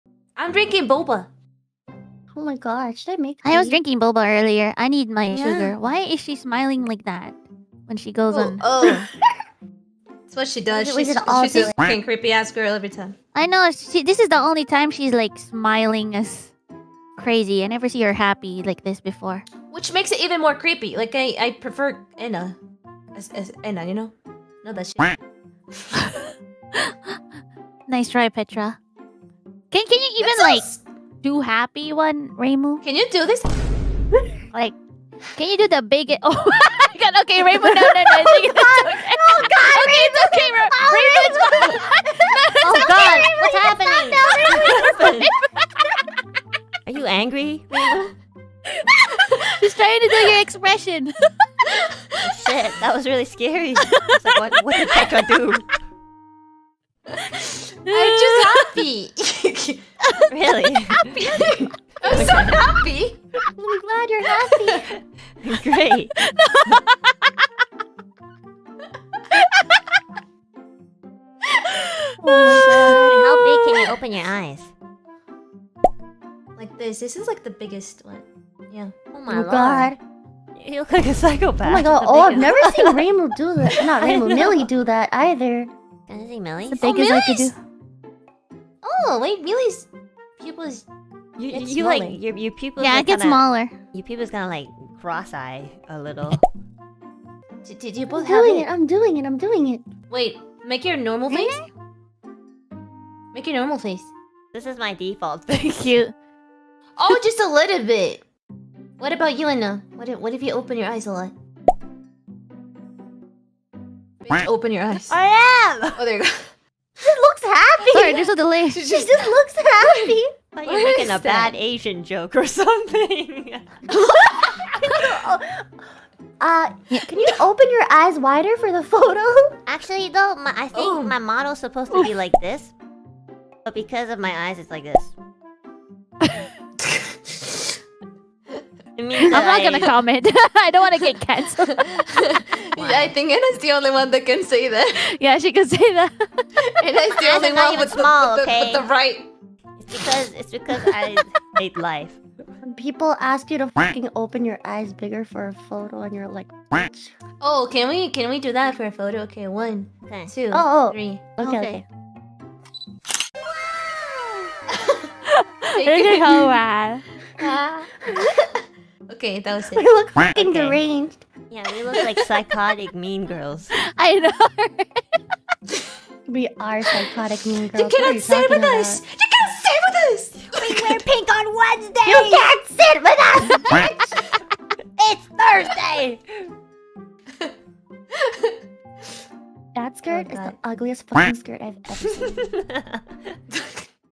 【NIJIUK】Reimu's Happy Face Scares the Girls【NIJISANJI EN | Enna+Reimu+Millie+Petra】
Source + Timestamp:
NIJIUK: GIRLS TALK ♡♡♡【NIJISANJI EN | Enna Alouette】